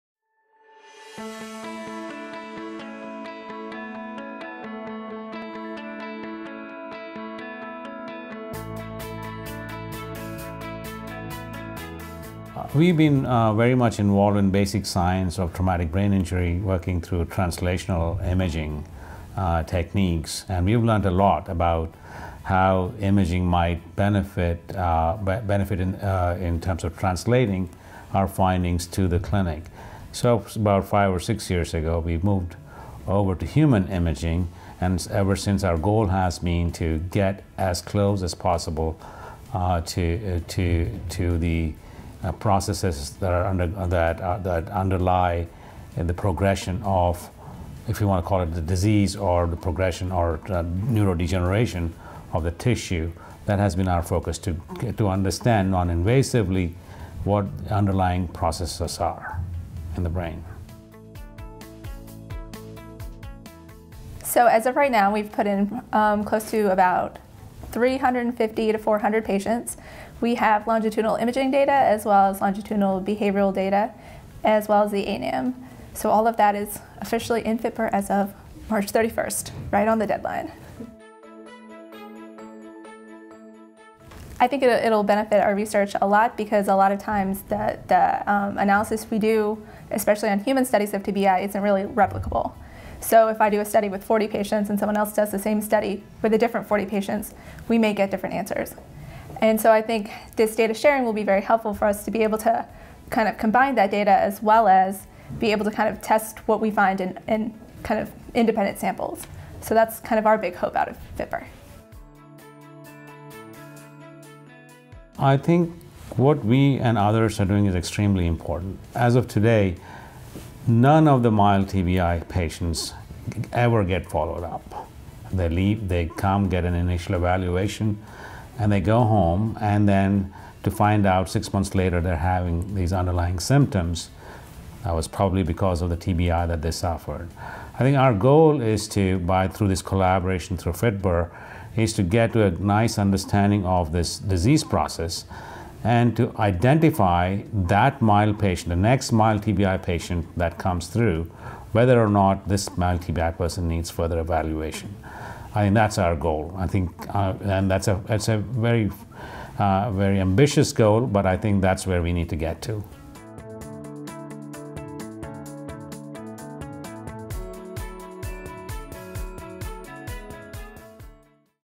Drs. Rao Gullapalli and Chandler Sours
Drs. Chandler Sours and Rao Gullapalli of the University of Maryland discuss their research examining imaging and behavior data to understand the processes underlying brain injury in mild TBI patients.